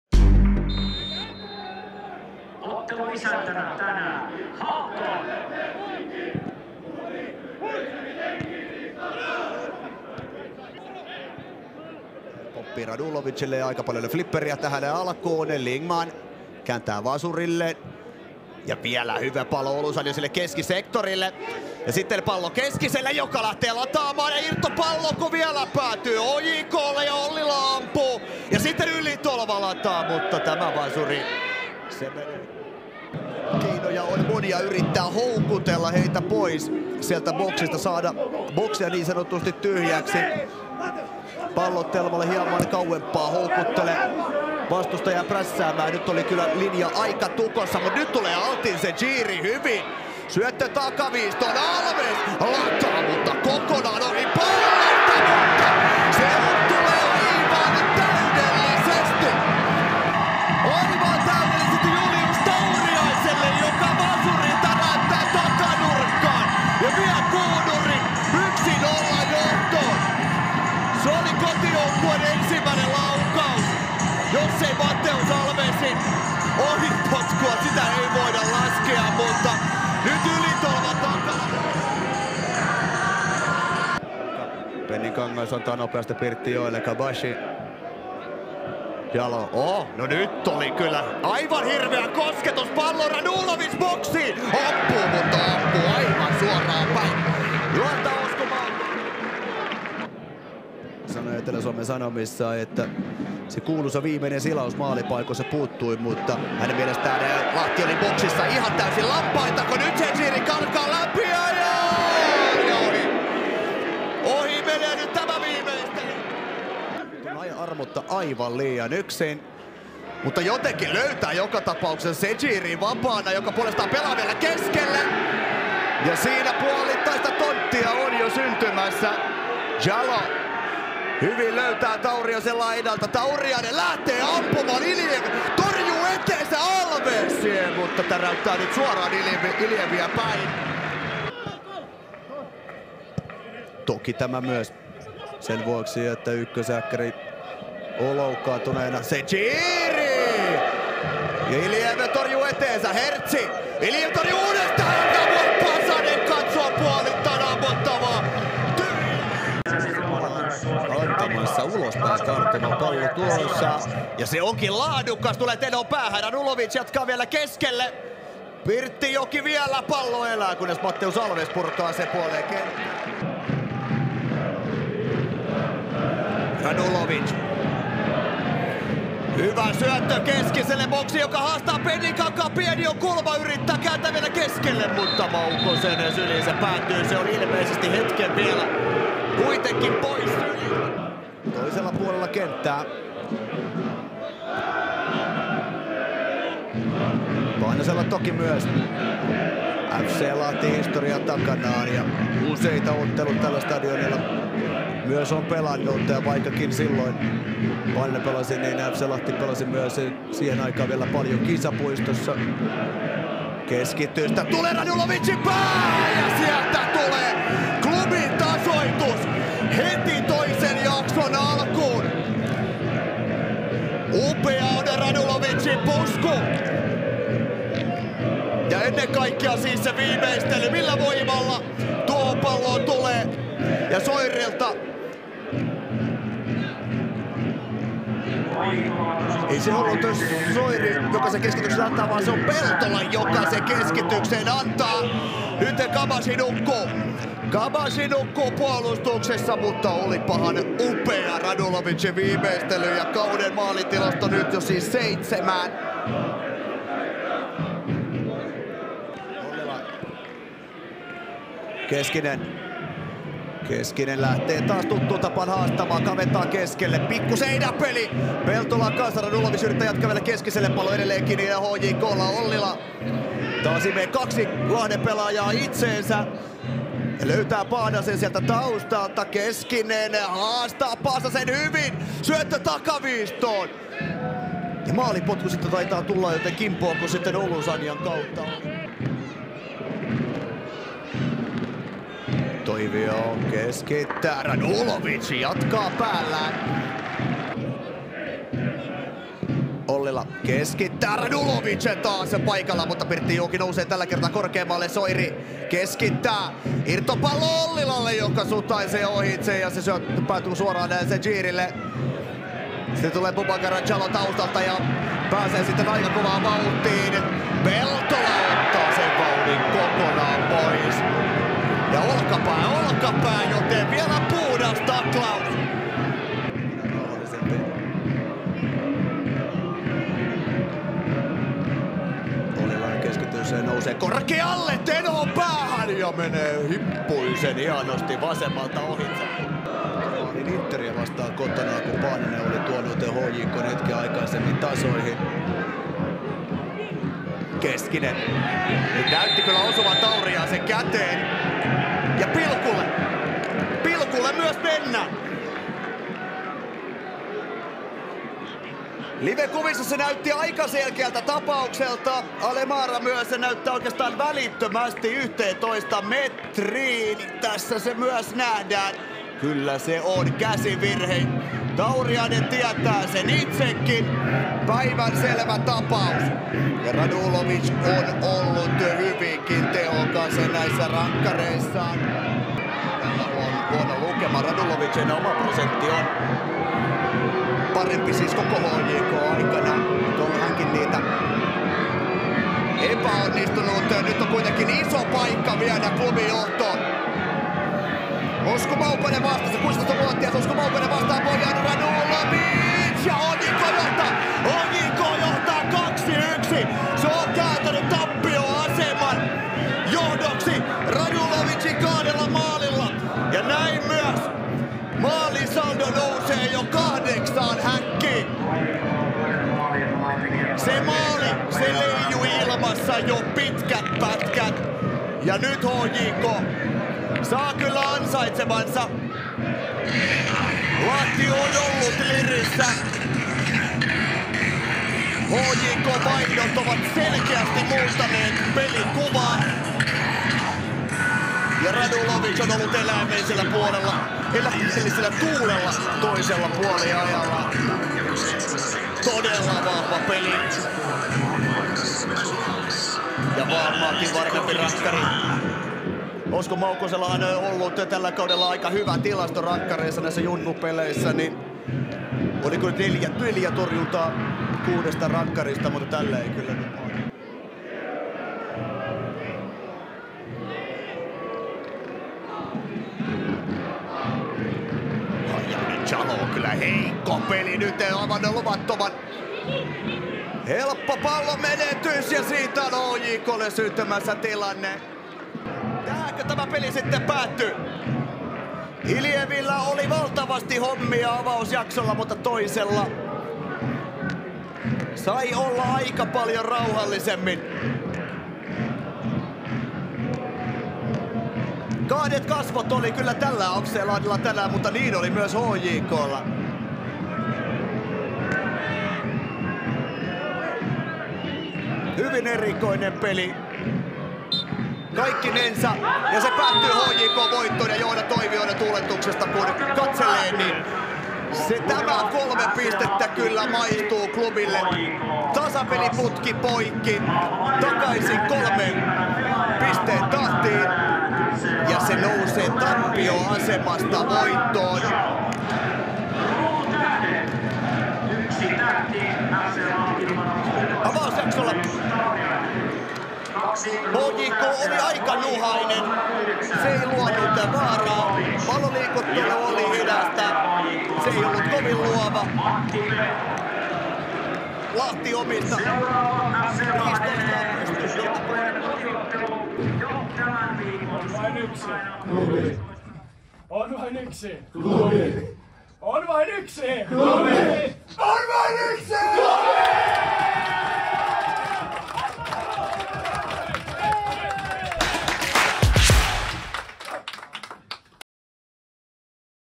FC Lahti vs HJK 1-2 – Veikkausliiga
7.6.2023 - Tärkeä voitto Lahdesta – Bojan Radulovic iski kahdesti. Klubi käänsi takaa-ajoaseman voitoksi toisella puoliajalla. Katso kooste ottelusta.